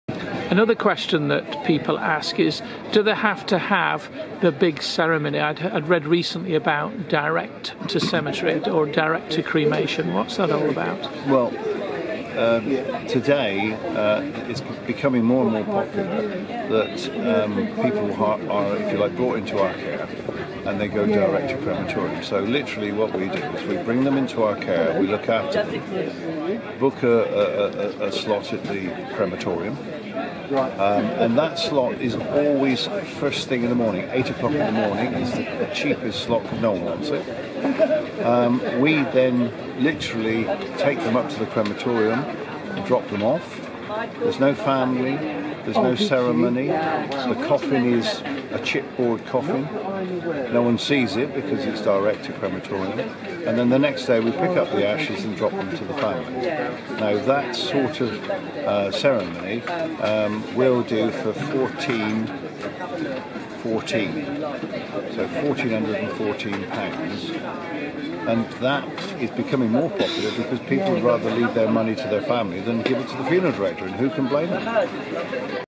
Direct to Crematorium
Funeral options - Direct to Crematorium. The cheapest funeral option is direct to crematorium according to Wells Funeral Services.